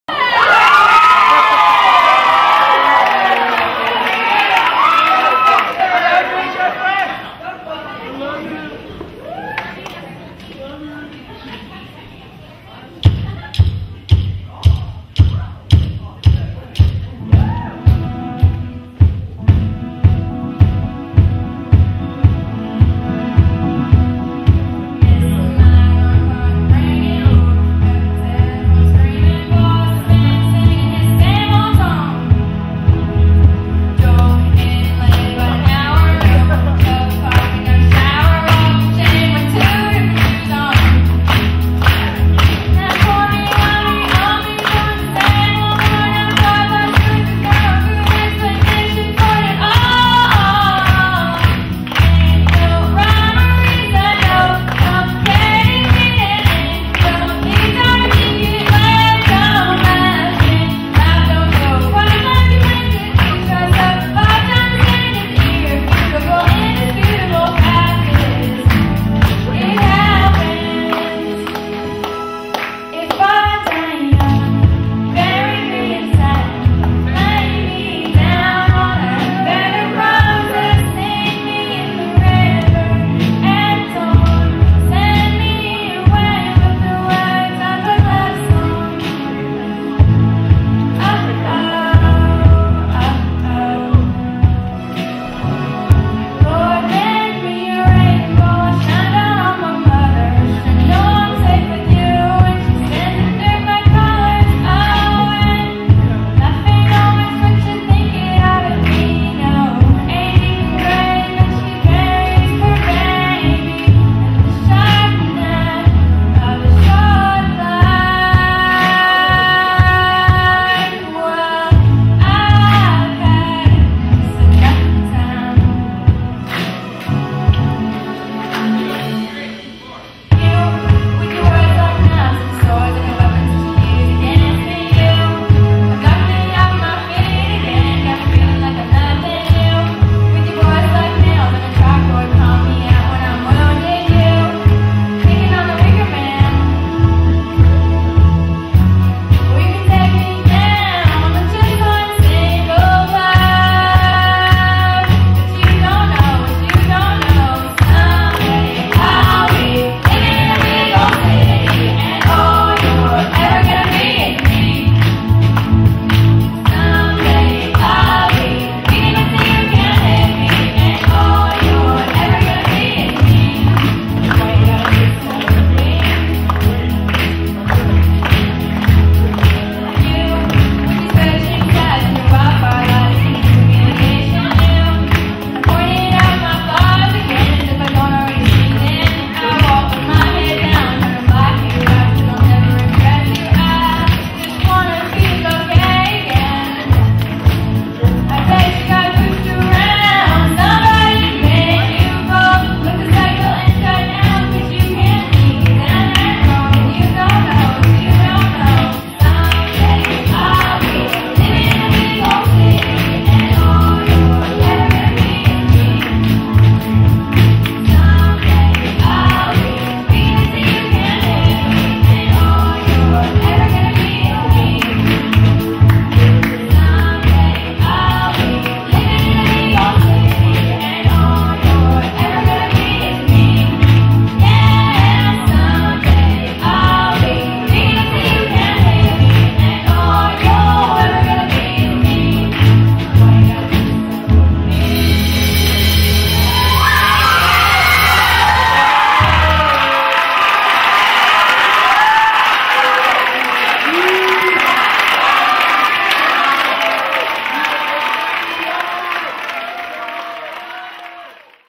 Mayada (13) and Jeffery (15) Medley in E major- Talent Show
Mayada put together a medley of 3 songs she likes for the school talent show. She was joined by Jeffery on keys and their friend Hannah on drums. Something a little different from the legit stuff. It was Mayada and Jeffery's first time to try pop music on stage. We all had fun. Given that we had to set up the stage in 2 minutes between acts, and Jeffery's keyboard didn't have the right sound *at all*, and the video camera mikes were way at the back of the packed auditorium and picked up a lot of junk, it turned out pretty well. It was a lot of fun.